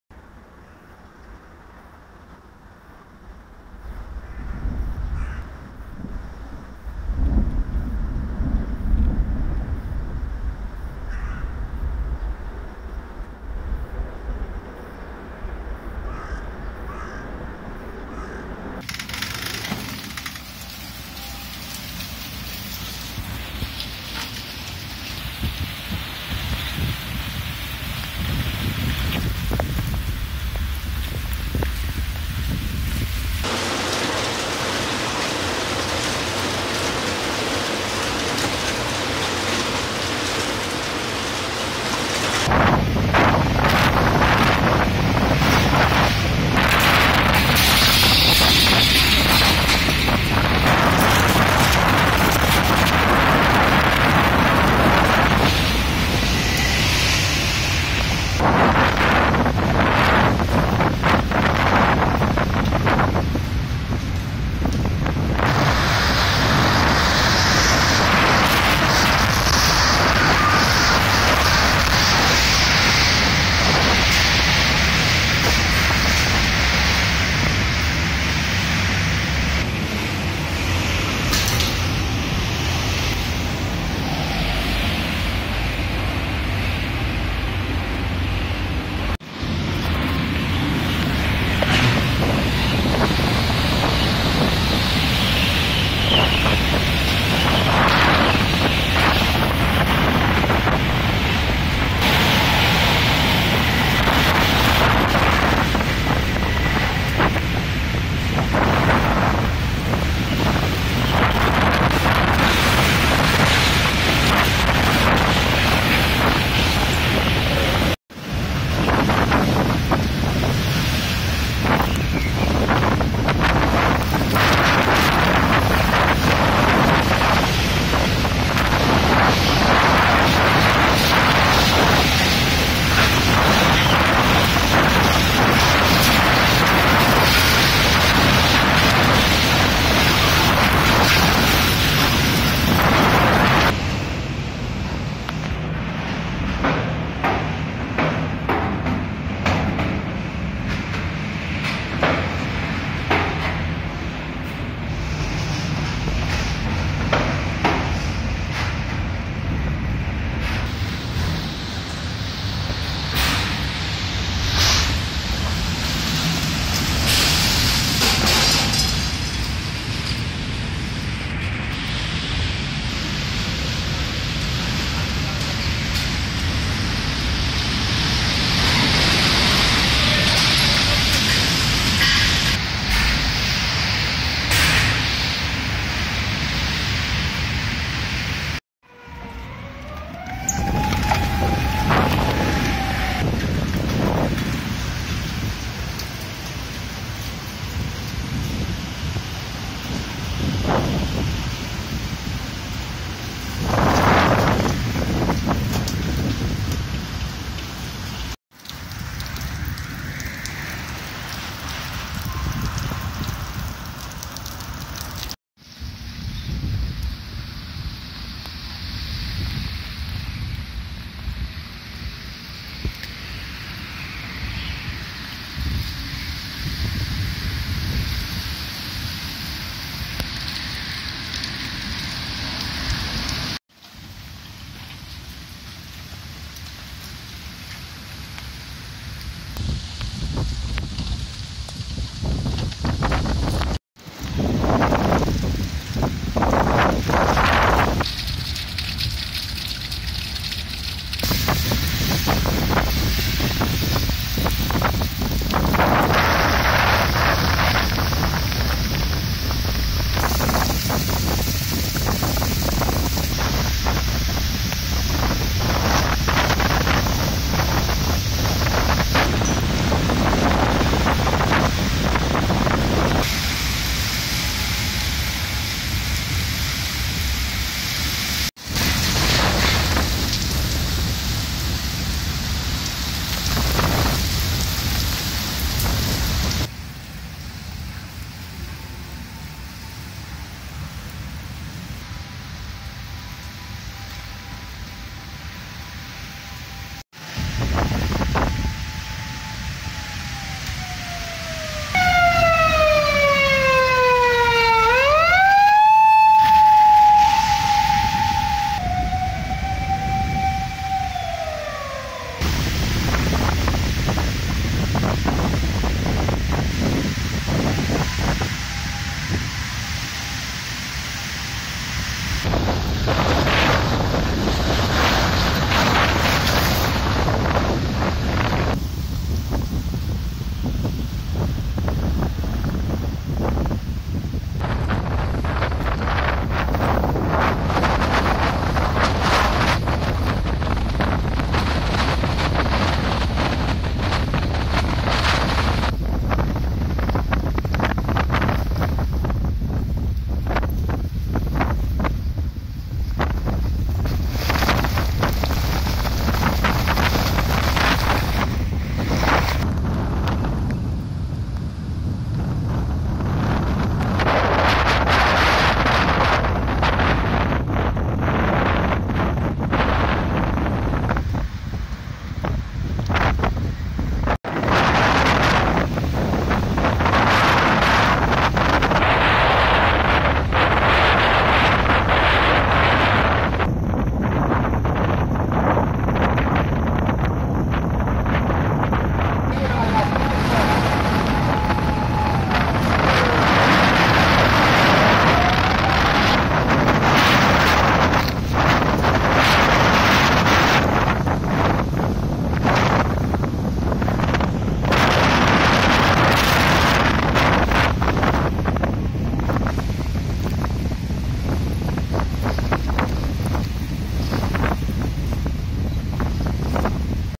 Typhoon Jebi - swept through Osaka today and wrecked a lot of stuff
The strongest winds I think I've ever experienced made a big mess of Osaka today, as Typhoon Jebi made landfall. It's typhoon season right now in Japan, but they're not always this strong.

I'm safe, although my flowerbox thing didn't make it. School was closed for the day but we'll see how it looks tomorrow...